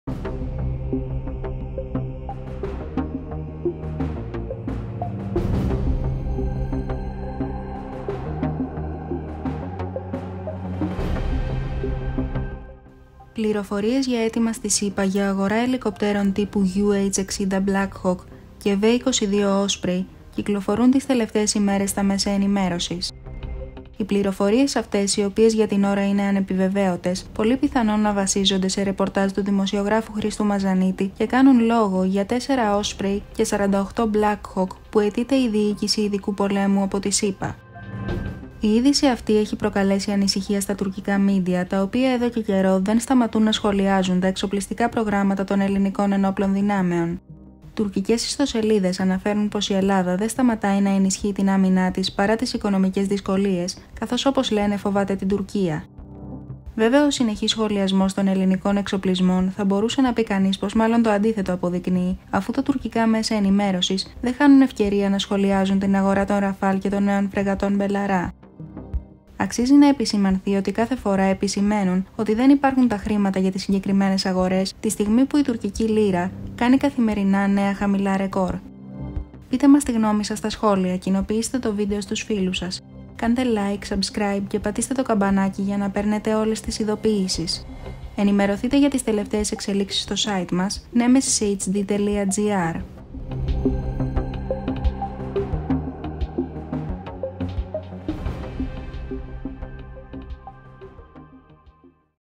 V22 Osprey & UH-60 Black Hawk: Ελληνικό και «Τουρκικό» ενδιαφέρον
📰 - ΕΙΔΗΣΕΙΣ NEMESIS HD - Πληροφορίες για αίτημα στις ΗΠΑ για αγορά ελικοπτέρων τύπου UH-60 Black Hawk και V-22 Osprey κυκλοφορούν τις τελευταίες ημέρες...Δες την είδηση

CHAPTERS

00:00​​​ INTRO
00:10​​​ ΕΙΔΗΣΗ
01:32​​​ OUTRO